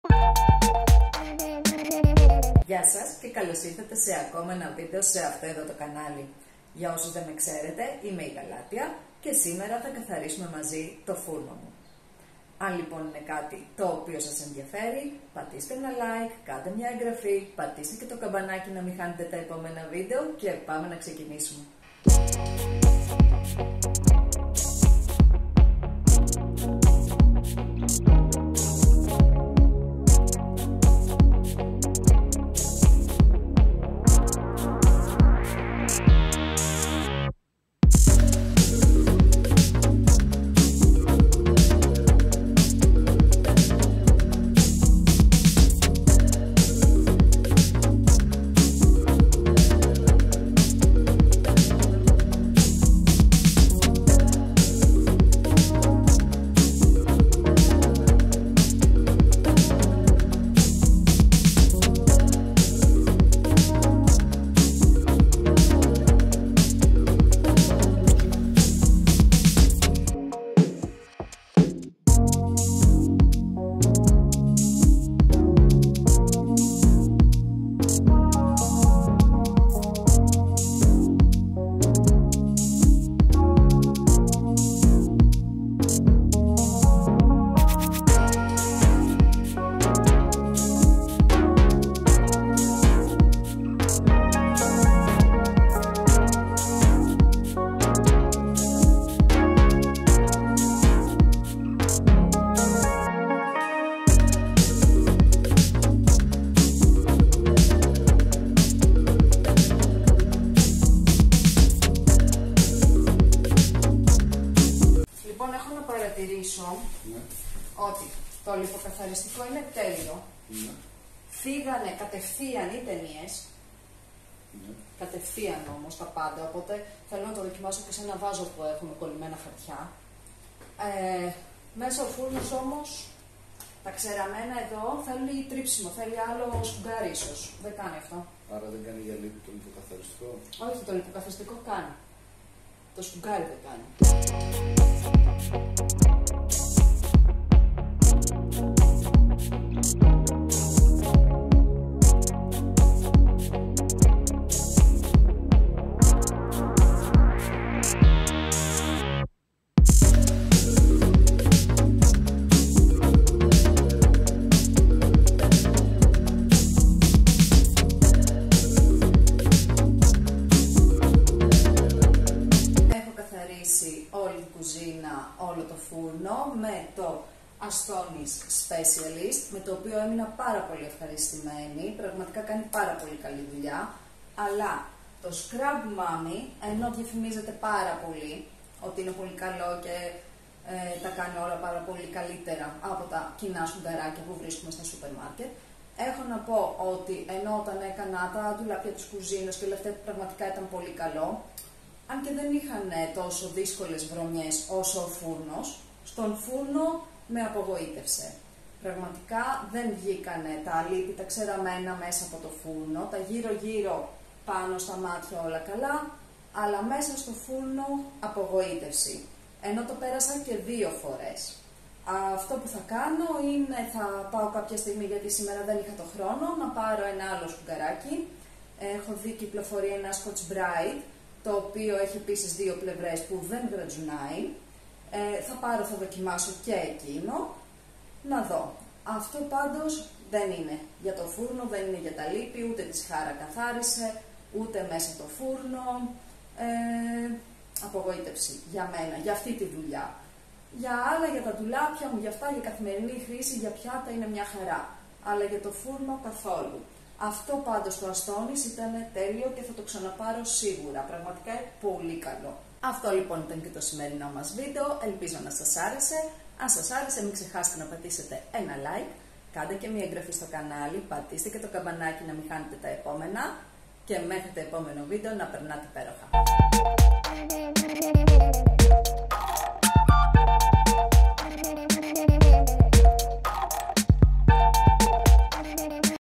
Καθαριότητα φούρνου | Galateia Taxidi
Στο σημερινό βίντεο έλα μαζί μου να καθαρίσουμε τον φούρνο μου. Ελπίζω αυτό το βίντεο να σου αρέσει και να σου φανεί πολύ χρήσιμο!

Μέχρι το επόμενο βίντεο, να περνάς υπέροχα 😘

🔔 Πάτα το καμπανάκι για να ενημερώνεσαι για τα νέα βίντεο